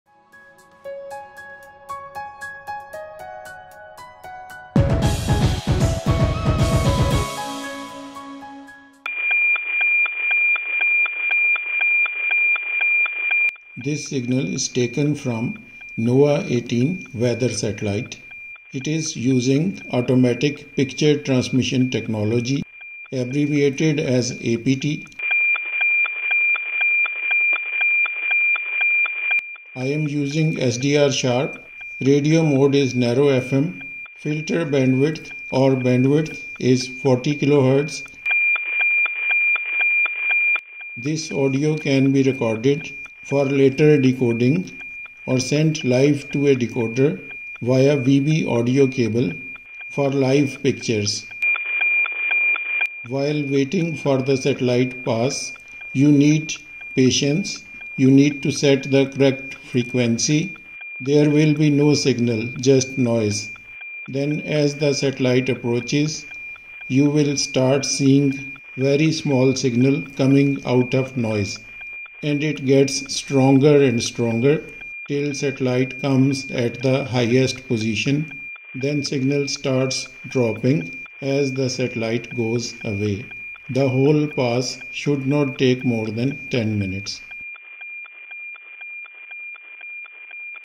Signal Identification of NOAA-18 Weather Satellite APT on RTL_SDR & SDR Sharp by Technology Master
Signal Identification of NOAA-18 Weather Satellite Automatic Picture Transmission APT Signal on RTL_SDR  &  Sharp.